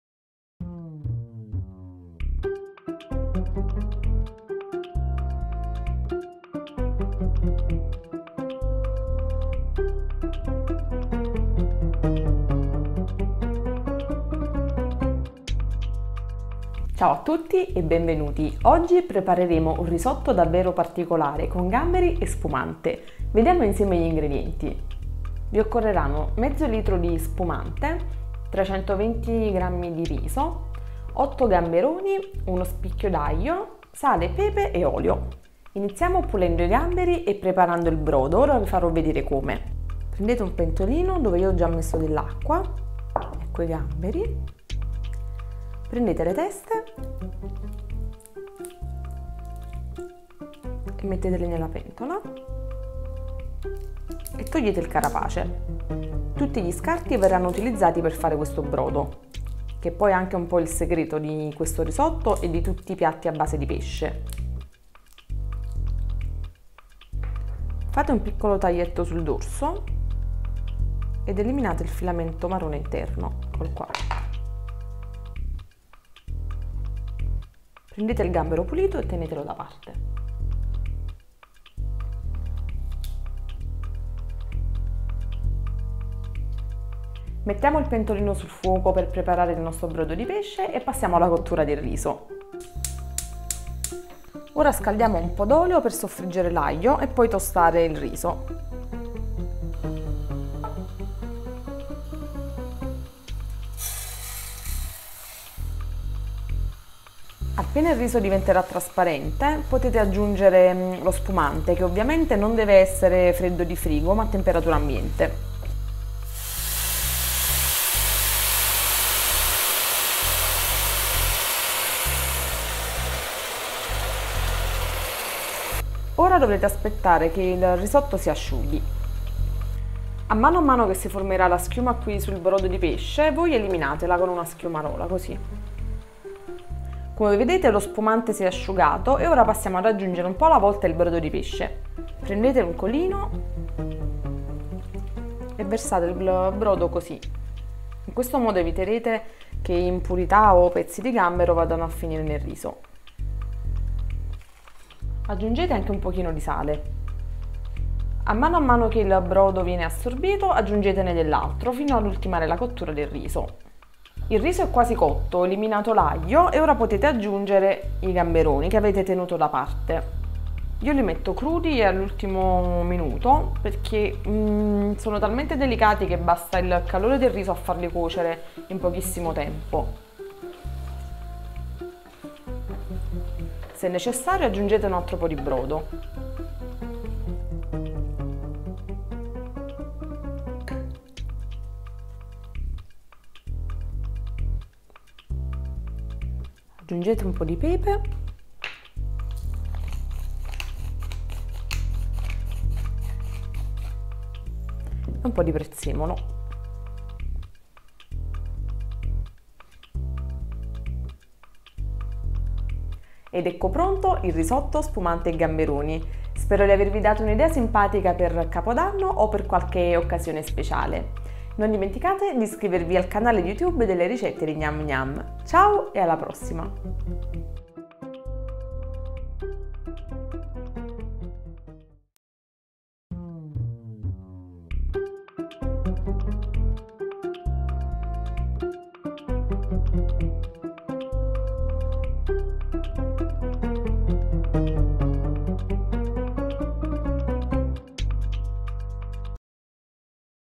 Risotto gamberoni e spumante - Gnam Gnam
Un primo elegante e d'effetto, per le occasioni speciali, come Capodanno o San Valentino!